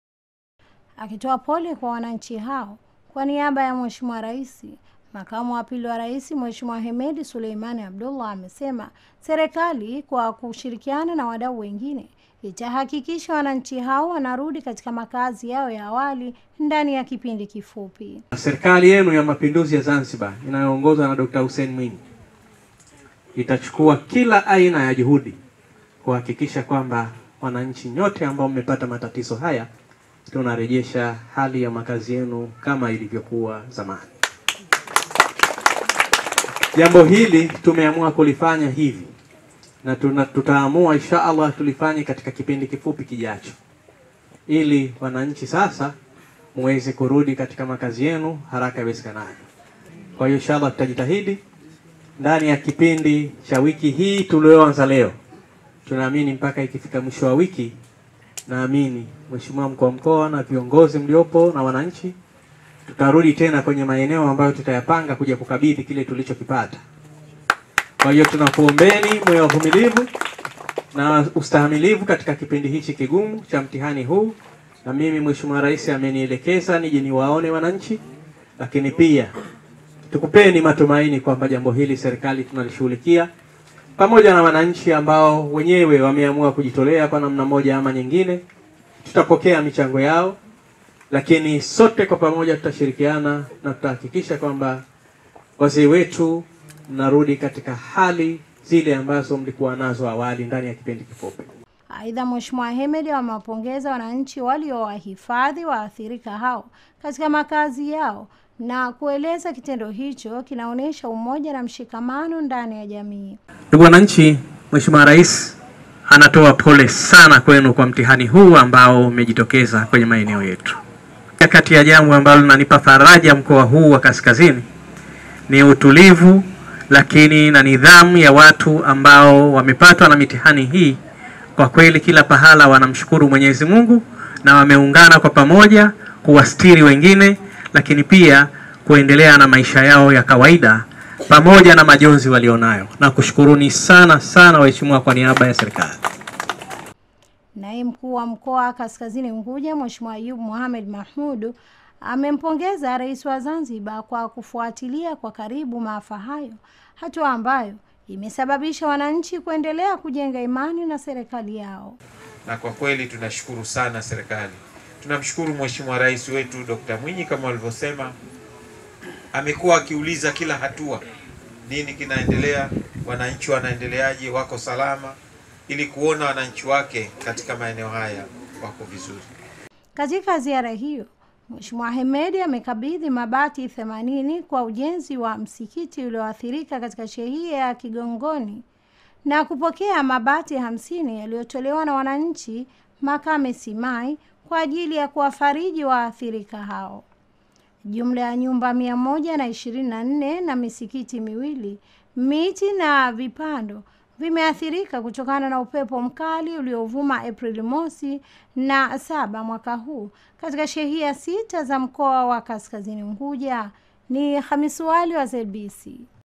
DK..MWINYI AMETOA POLE KWA WANANCHI WALIOATHIRIKA NA UPEPO MKOA WA KASKAZINI UNGUJA.
Akitoa pole kwa Wananchi hao kwa Niaba ya Rais Makamu wa pili wa Rais Mhe. Hemed Suleiman Abdalla amesema Serikali kwa kushirikiana na Wadau wengine itahakikisha Wananchi hao wanarudi katika Makazi yao ya awali ndani ya kipindi kifupi.

Aidha Mhe. Hemed amewapongeza Wananchi waliowahifadhi waathirika hao katika makazi yao na kueleza kitendo hicho kinaonesha umoja na mshikamano ndani ya Jamii.

Nae Mkuu wa Mkoa wa Kaskazini Unguja Ayoub Muhamed Mahmoud amempongeza rais wa Zanzibar kwa kufuatilia kwa karibu maafa hayo hatua ambayo imesababisha Wananchi kuendelea kujenga  imani na Serikali yao.

Katika ziara hiyo Mhe.Hemed amekabidhi Mabati 80 kwa ujenzi wa Mskiti ulioathirika katika Shehia ya Kigongoni na kupokea Mabati 50 yaliyotolewa na Mwananchi makame Simai kwa ajili ya kuwafariji Waathirika hao.

Jumla ya Nyumba mia moja na 24 na Misikiti miwili, Miti na Vipando vimeathirika kutokana na upepo Mkali uliovuma Aprili Mosi na Saba mwaka huu katika Shehia sita za Mkoa wa Kaskazini Unguja.